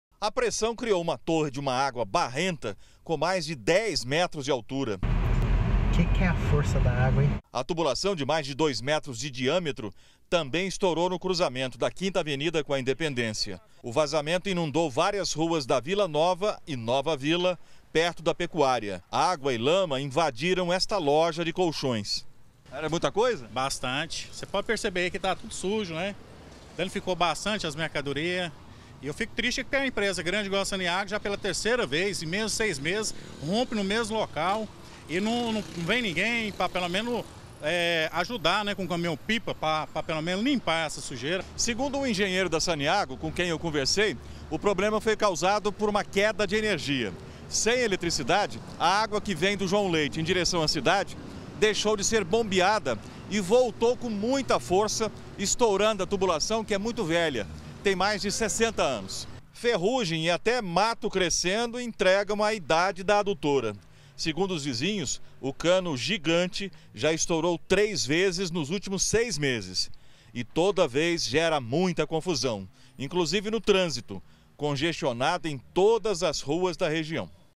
GR - Imagem impressionante: Adutora se rompe e causa estragos - 01-03-2018
Uma adutora da SANEAGO arrebentou hoje na Região Leste de Goiânia. Gerou prejuízo e transtornos. Várias ruas ficaram alagadas. a água entrou até em algumas lojas. A imagem da água jorrando é impressionante. Tanto pela quantidade quanto pela cor.